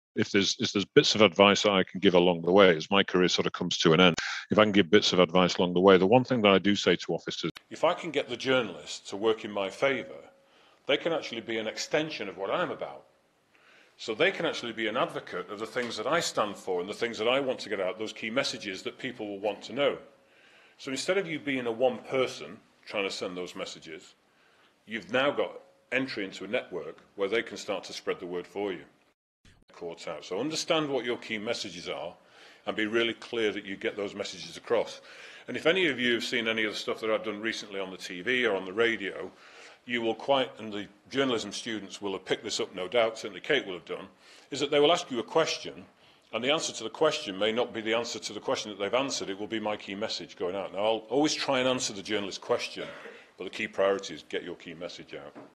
Nick Adderley's views on using journalists to get his "key message" out.
In a poetic bit of foreshadowing, Nick Adderley talks about giving advice as his career comes to an end. Given how bad a liar he is and the criminal charges he is facing, I am not sure how much stock we should put in his advice, but here we are.

In a composite of 2 different interview clips to add the career ending part in for shits and giggles, we focus on an interview post Covid where he sticks 2 fingers up at the public, by brazenly saying how he used journalists to get his "key message" across and how he tried to give politicians answers to questions.

The most notable example of this was how when journalist John Scheerhout published his book in 2013, accepting that Adderley as someone born in 1966, served in the Falklands as a Lieutenant in 1982 without fact checking any of it.

This use of a journalist writing this lie in a book, amplified Nick's impossible claims and enabled his Walting to go nationwide. By his own admission, the journalists can be an extension of him and instead of being a single person, this gave him entry into a network who could spread the word for him.